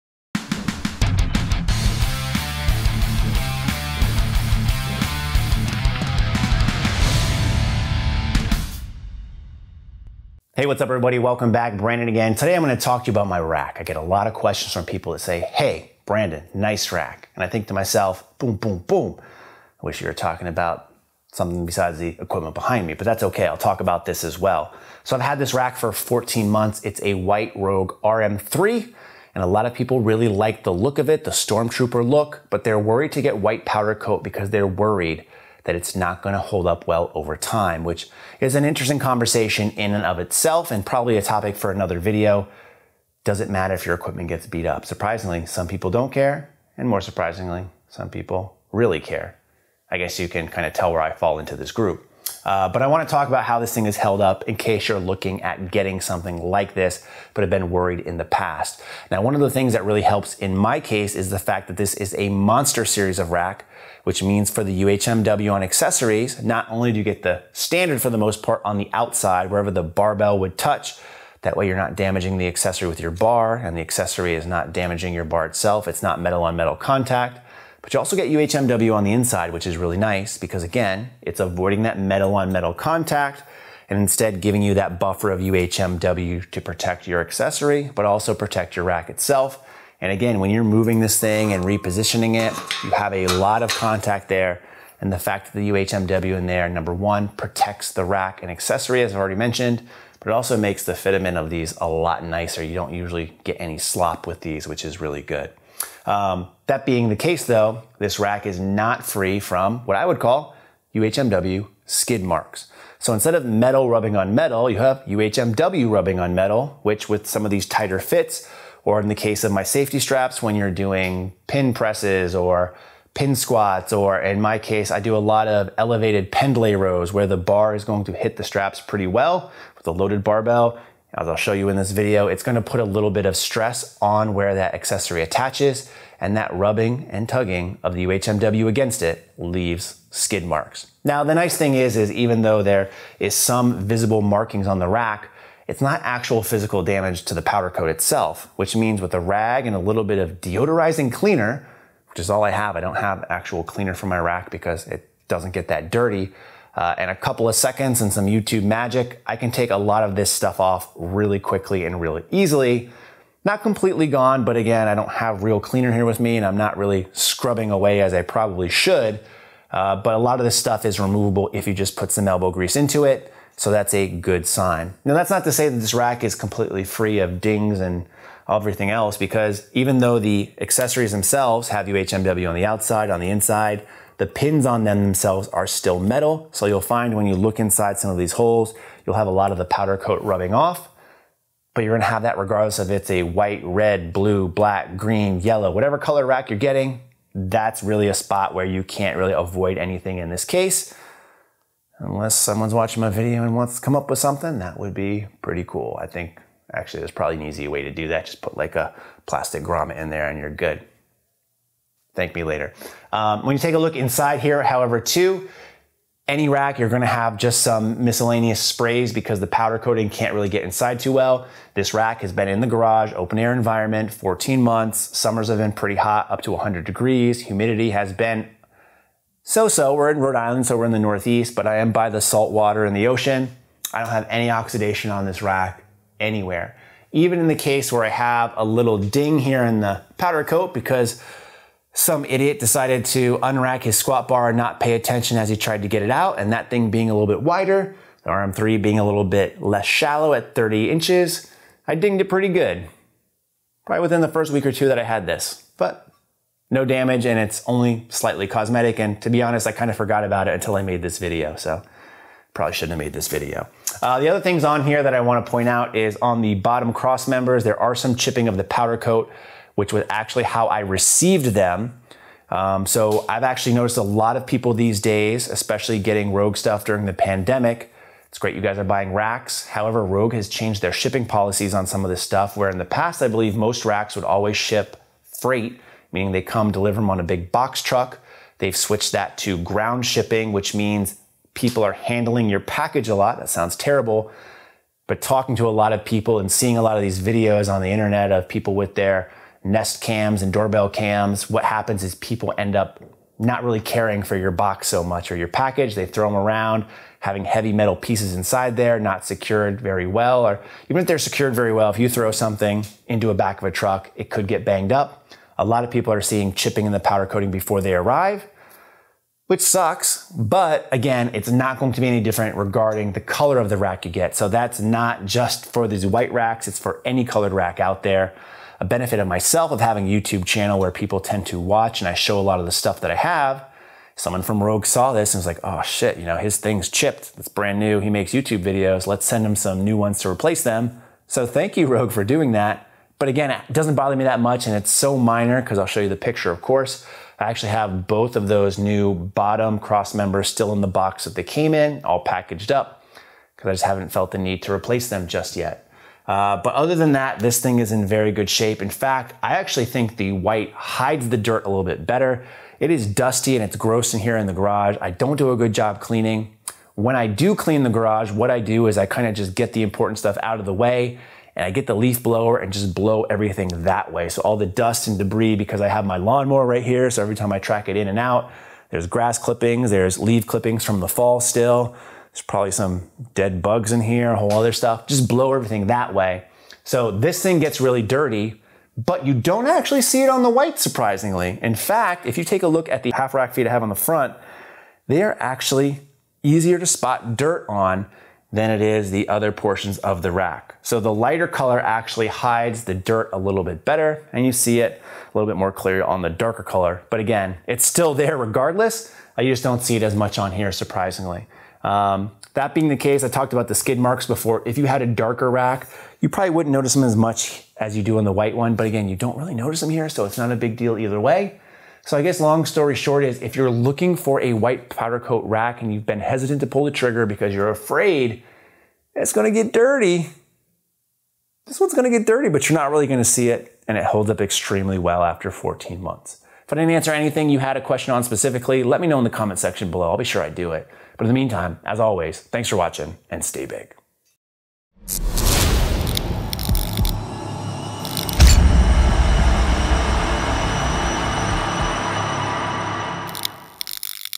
Rogue RM-3 White Powder Coat Power Rack - 1 Year Later
I get a lot of compliments on my rack (hey now), but also a lot of people who say they're hesitant to pull the trigger b/c they fear the wear and tear on white powder coat will show more. In this video I talk about my experiences over the past 14 months.